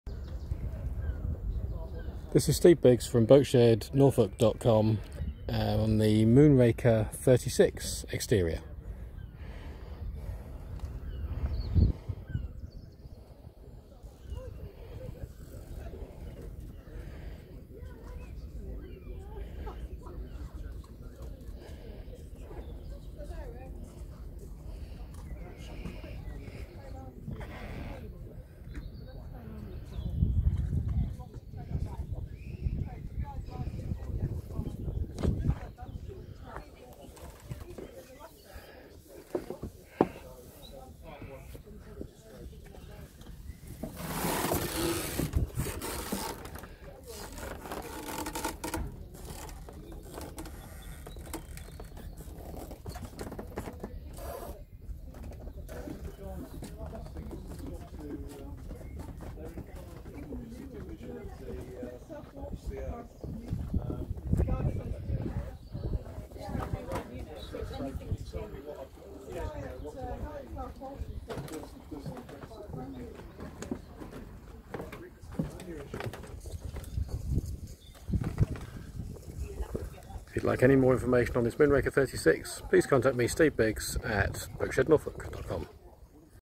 Moonraker 36 Softrider - Boatshed - Boat Ref#301413
Moonraker 36 Softrider for sale with Boatshed Norfolk Photos and video taken by Boatshed Norfolk